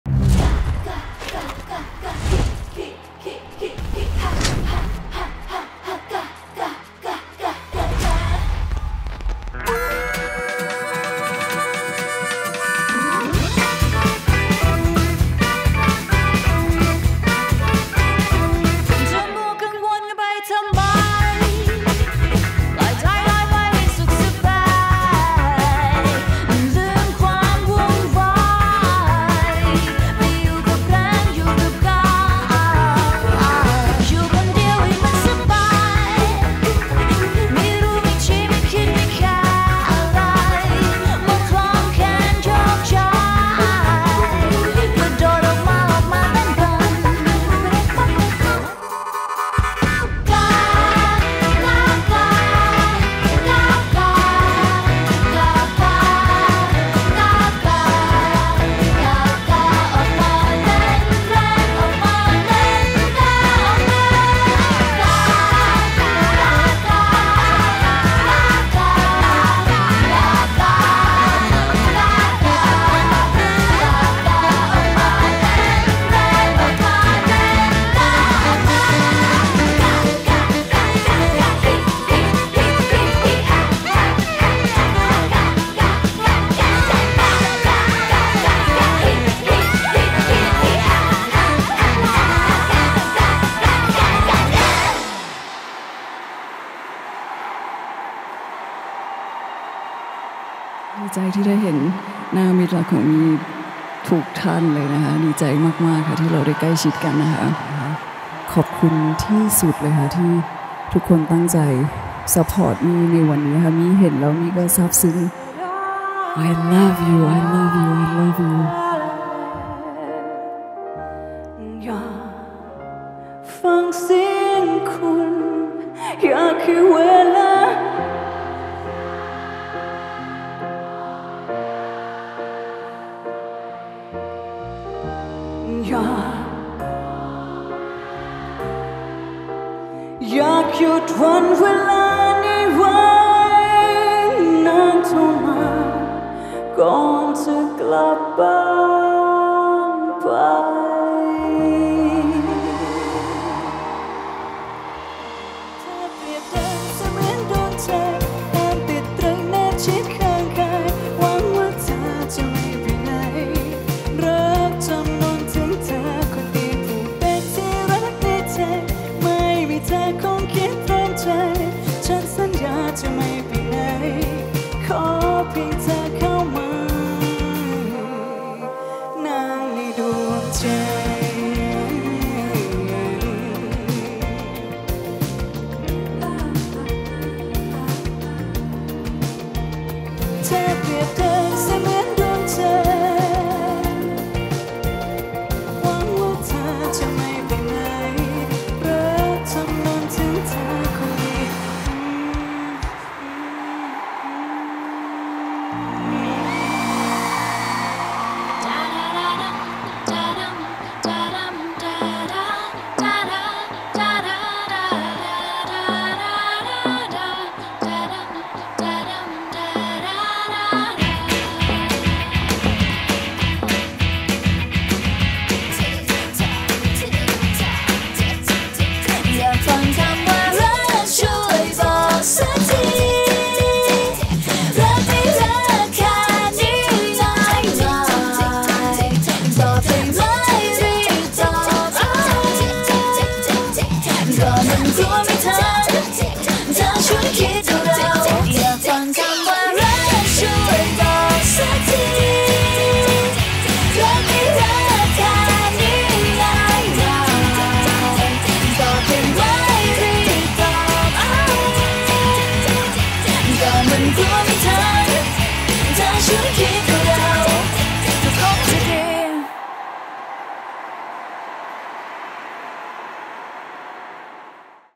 PALMY มิตร Universe Concert l Aftermovie
ขอขอบพระคุณมิตรรักทุกท่าน
ทั้ง 4 รอบการแสดงจากดวงใจครับ
🎥 Krungthai-AXA Life presents
PALMY มิตร Universe Concert
วันที่ 7, 8, 11, 12 กันยายน 2567
ณ อิมแพ็ค อารีน่า เมืองทองธานี